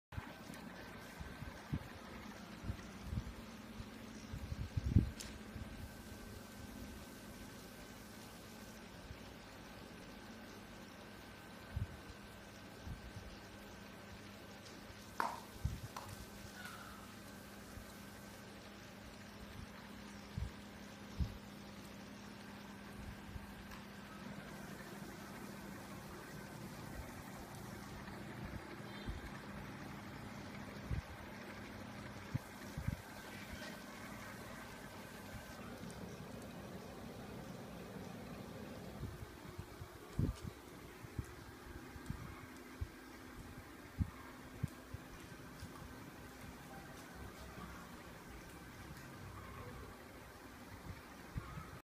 AMật - Cái tội dám quấy rối chị tôi này.
Chuyện buổi chiều của Mật:
Khi mà tôi bị 1 con ong bé nhỏ bay xung quanh người khi đang ngồi chơi đt.
Ngay lập tức tôi quay sang nói với AMật rằng: "Á Mật, có con gì kìa" rồi xong và đây là kết quả..